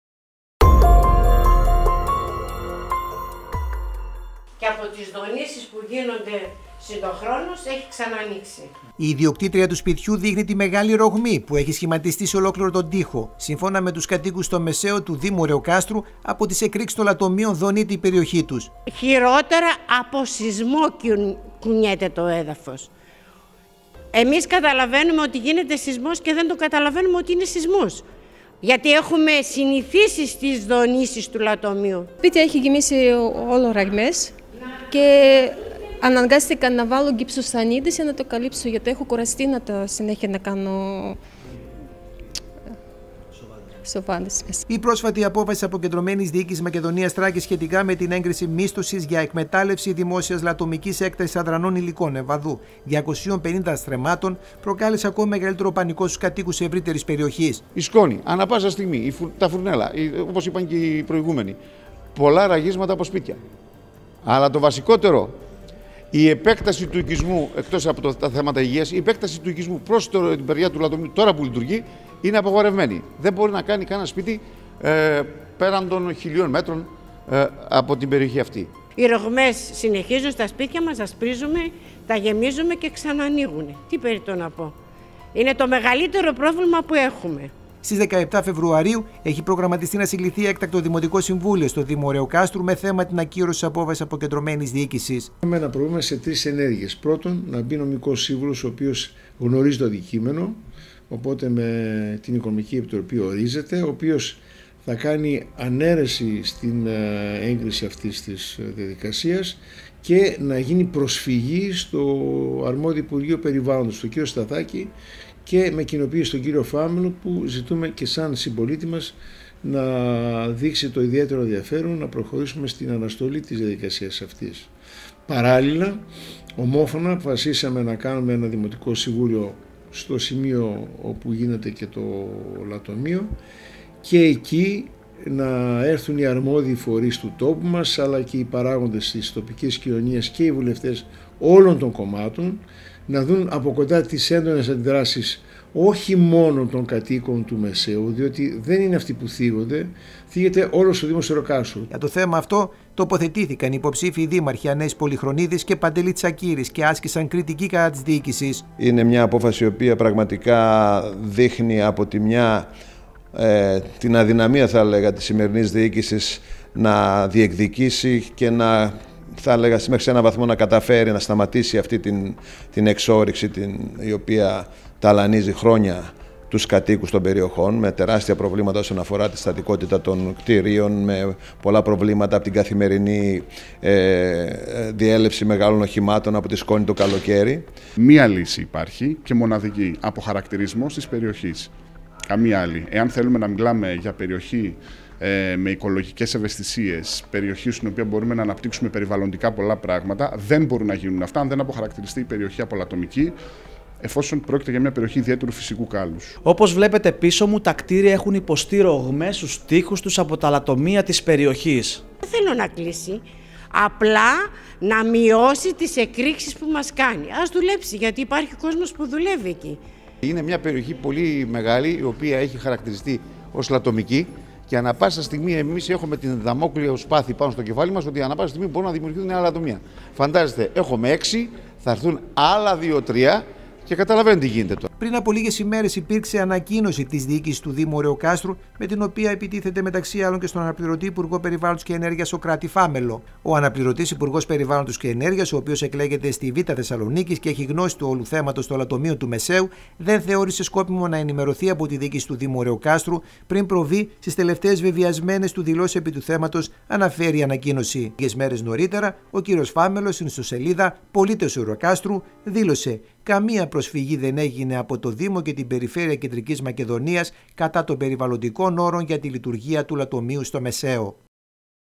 Η ΕΠΕΚΤΑΣΗ ΤΗΣ ΛΑΤΟΜΙΚΗΣ ΠΕΡΙΟΧΗΣ ΠΡΟΚΑΛΕΙ ΑΝΗΣΥΧΙΑ
Ανάστατοι είναι οι κάτοικοι του Μεσαίου του Δήμου Ωραιοκάστρου για τις ρηγματώσεις που έχουν δημιουργηθεί μέσα στα σπίτια τους αλλά και για την επιβάρυνση του υδροφόρου ορίζοντα από τη λειτουργία των λατομείων. Την ανησυχία των κατοίκων επέτεινε ακόμη περισσότερο  η απόφασης της αποκεντρωμένης διοίκησης Μακεδονίας – Θράκης σχετικά με την “έγκριση μίσθωσης για εκμετάλλευση δημόσιας λατομικής έκτασης αδρανών υλικών εμβαδού 250 στρεμμάτων ”.  Οι κάτοικοι ανησυχούν για το γεγονός ότι δίνεται η δυνατότητα για την επαναλειτουργία του λατομείου για διάστημα 20 ετών.

Στις 17 Φεβρουαρίου έχει προγραμματιστεί  να συγκληθεί  έκτακτο δημοτικό συμβούλιο στον δήμο Ωραιοκάστρου, με θέμα την ακύρωση  της απόφασης της Αποκεντρωμένης. Για το συγκεκριμένο θέμα ξέσπασε πόλεμος μεταξύ των υποψηφίων, ενώ υπήρξε  ανακοίνωση  του δήμου με αιχμές κατά του αναπληρωτή  Υπουργού Περιβάλλοντος και Κλιματικής Αλλαγής, Σωκράτη Φάμελλο.